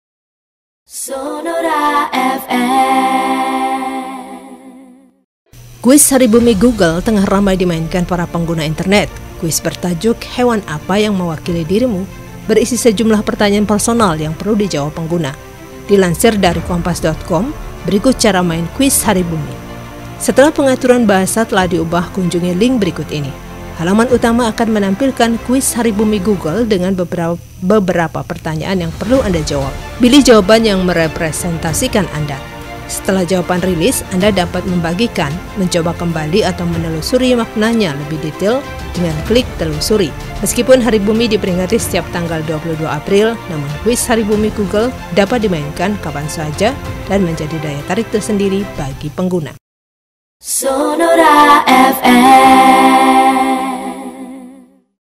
Jangan Bingung, Begini Cara Main Kuis Hari Bumi di Google | Sonora ID
Kuis Hari Bumi Google tengah ramai dimainkan para pengguna internet. Kuis bertajuk Hewan Apa yang Mewakili Dirimu? berisi sejumlah pertanyaan personal yang perlu dijawab pengguna. Dilansir dari kompas.com, berikut cara main kuis hari bumi.

Setelah pengaturan bahasa telah diubah kunjungi link berikut ini Halaman utama akan menampilkankuis Hari Bumi Google dengan beberapa pertanyaan yang perlu Anda jawab. Pilih jawaban yang merepresentasikan Anda

Setelah jawaban rilis, Anda dapat membagikan, mencoba kembali, atau menelusuri maknanya lebih detail dengan klik "Telusuri". Meskipun Hari Bumi diperingati setiap tanggal 22 April, namun kuis Hari Bumi Google dapat dimainkan kapan saja dan menjadi daya tarik tersendiri bagi pengguna.